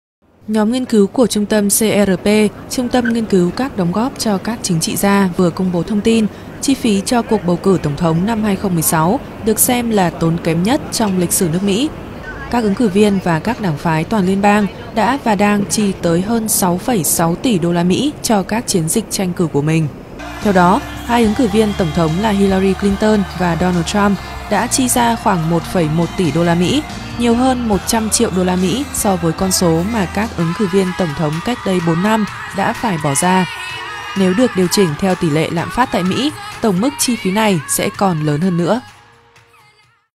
Cuộc bầu cử tổng thống Mỹ 2016 tiêu tốn hơn 6 tỷ USD
Cuộc bầu cử tổng thống năm 2016 có lẽ là cuộc chạy đua vào Nhà Trắng tốn kém nhất trong lịch sử nước Mỹ khi các ứng cử viên và các đảng chính trị ước tính đã chi hơn 6,6 tỷ USD, cao hơn hẳn so với các cuộc bầu cử trước đó.

Ứng viên tổng thống đảng Dân chủ Hillary Clinton và đối thủ của bà bên phía đảng Cộng hòa, tỷ phú Donald Trump, đã chi hơn 1,13 tỷ USD, nhiều hơn 100 triệu USD so với số tiền mà các ứng viên tổng thống trong mùa bầu cử năm 2012 đã phải bỏ ra./.